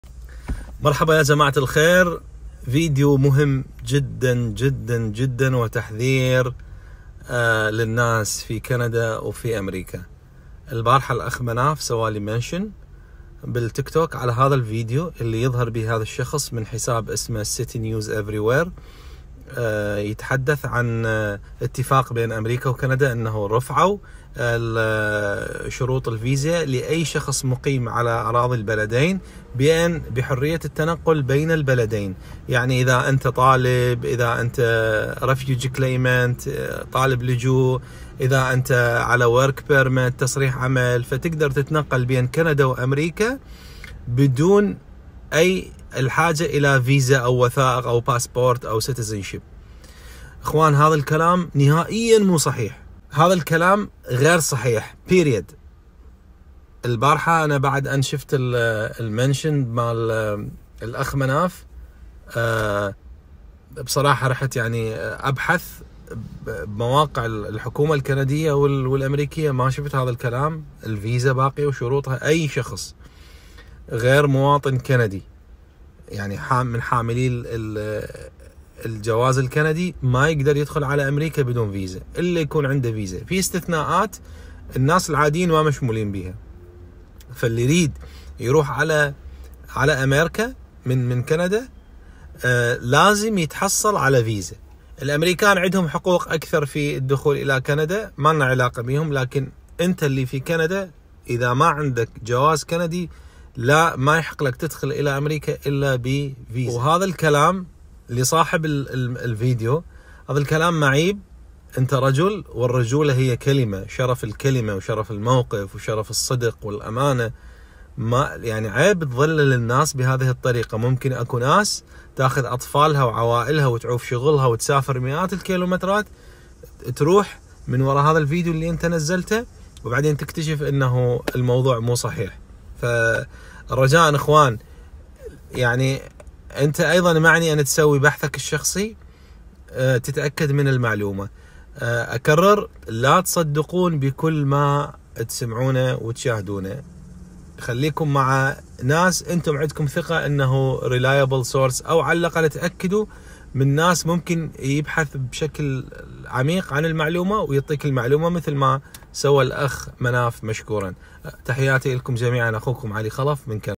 تحذير من محتوى فيديو كاذب إنتشر مؤخراً عن الغاء شرط الفيزا بين كندا وامريكا لغير حاملي الجواز الكندي
إنتباه تحذير من محتوى فيديو كاذب إنتشر مؤخراً عن الغاء شرط الفيزا بين كندا وامريكا لغير حاملي الجواز الكندي.